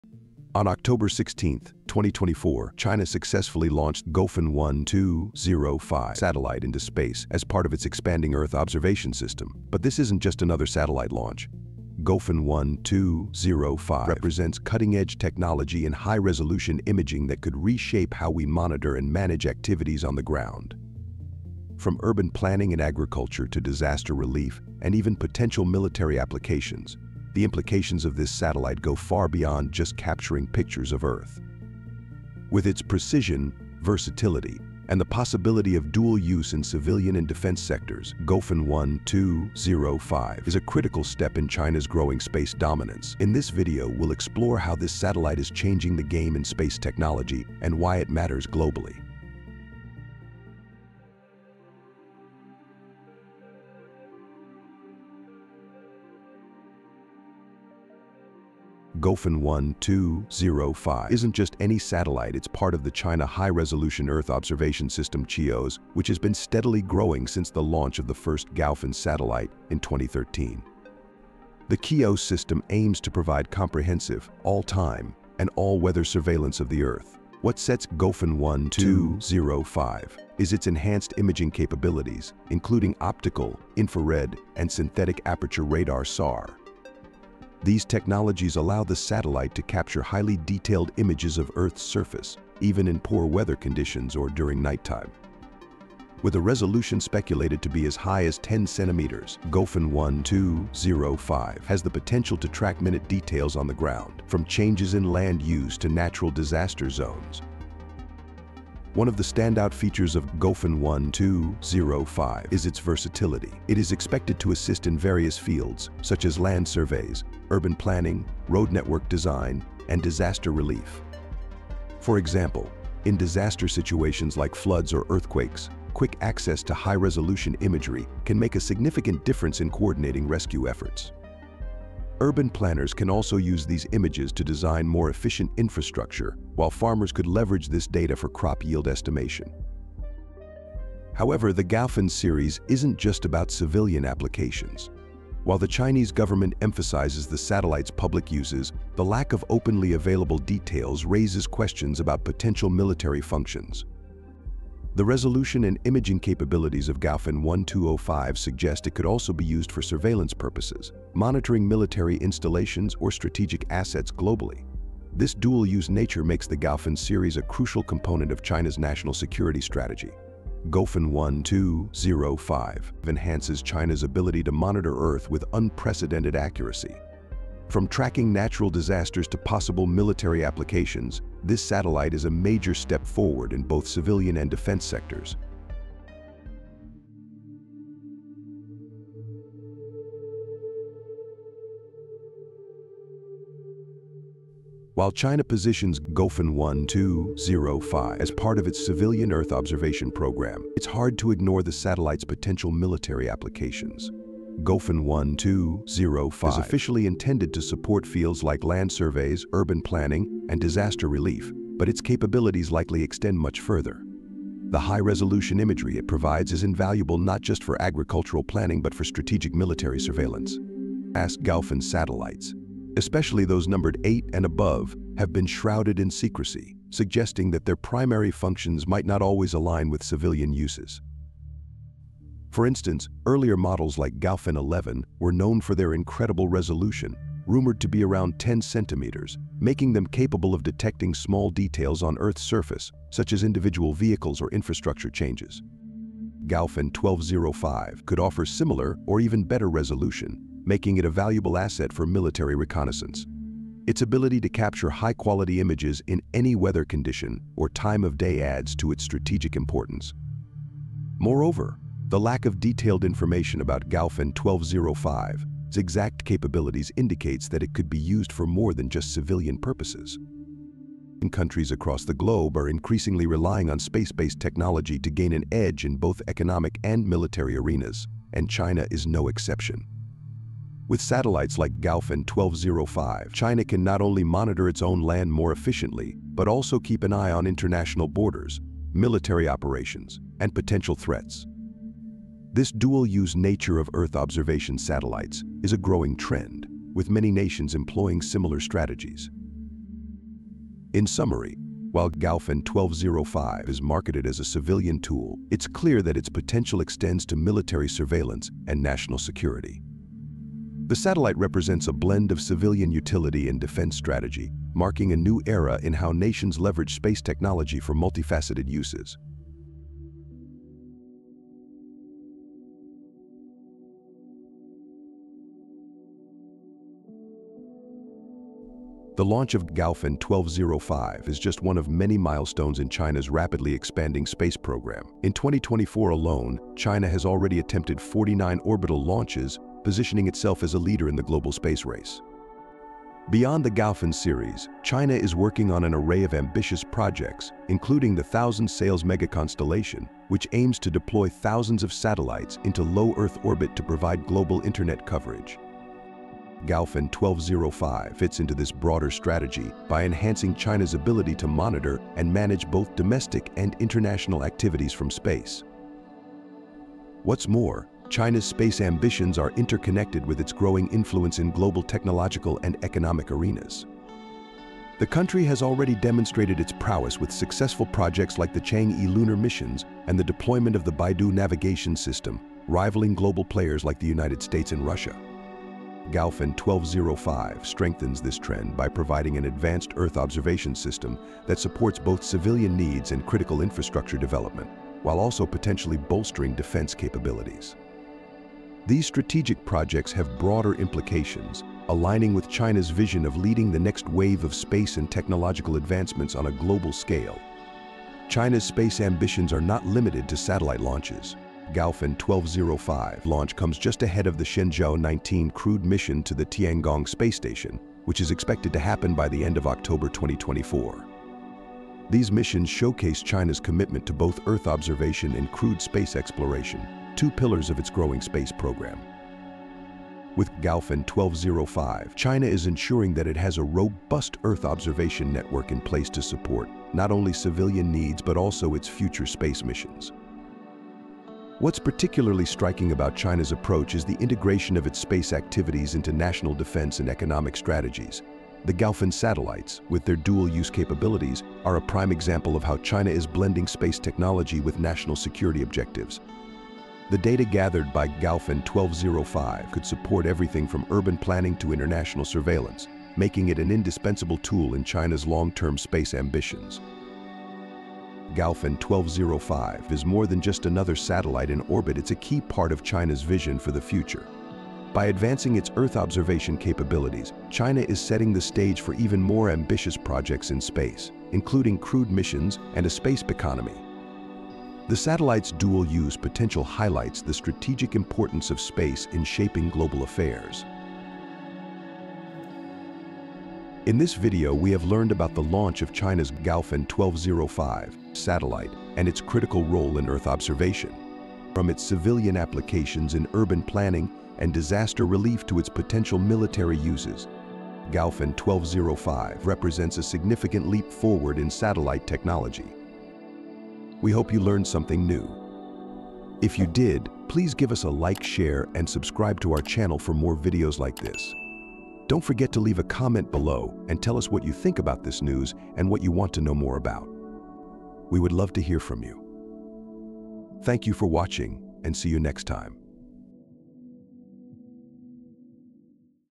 Breaking: China Launches New Gaofen-12 Satellite – More Than Just Earth Observation!
China has just launched its Gaofen-12 (05) satellite, adding to its growing fleet of Earth observation satellites. This high-resolution satellite is designed for everything from land surveys and urban planning to disaster relief, but its potential dual-use for national security is what’s raising eyebrows. With its advanced imaging technology, Gaofen-12 (05) is part of China’s broader ambitions to dominate space exploration and Earth monitoring. In this episode, we break down what this satellite does, how it fits into China’s space program, and why it’s a big deal.